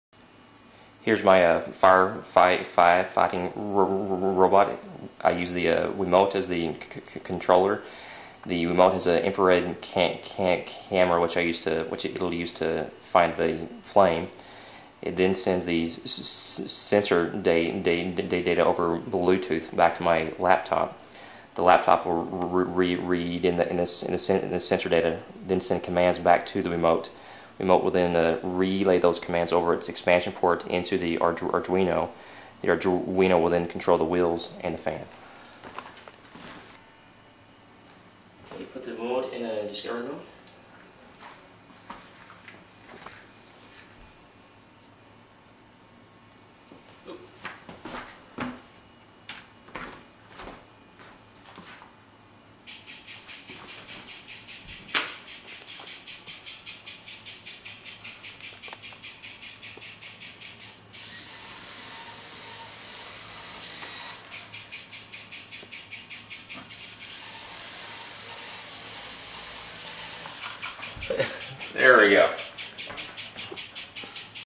Wiimote firefighting robot
Fire fighting robot built around a wiimote.  It still has bugs, but shows what the wiimote is capable of.